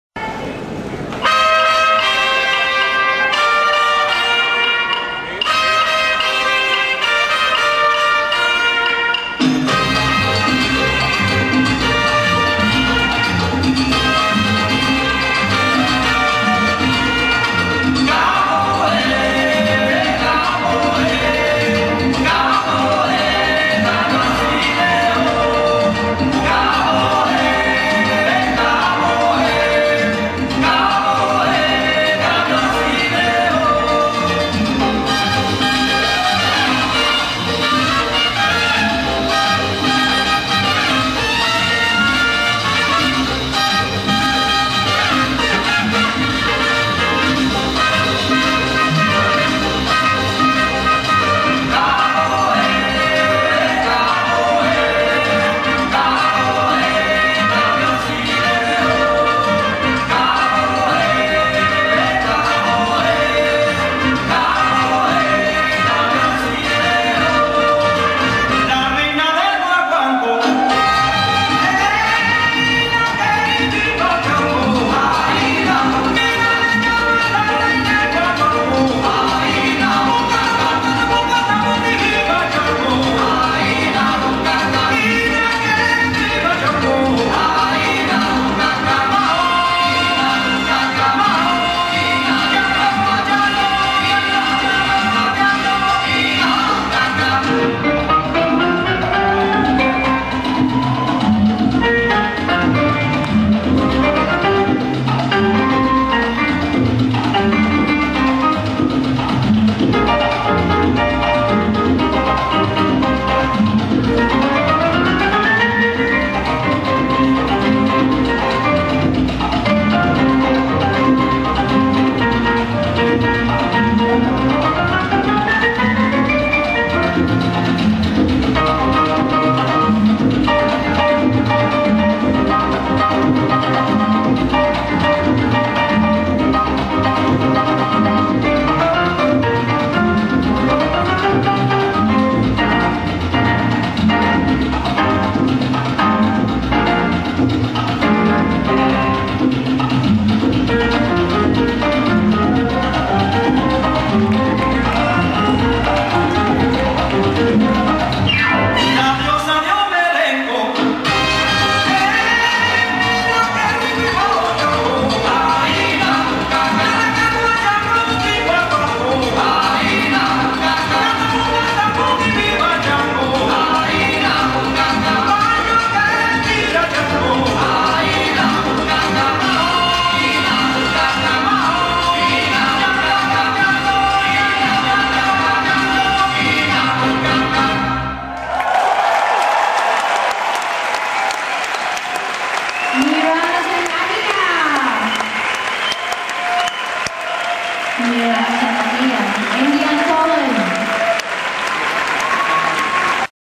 Neeraj & Ania Performance
This is performance of Neeraj and Ania at Hamburg Salsa Congress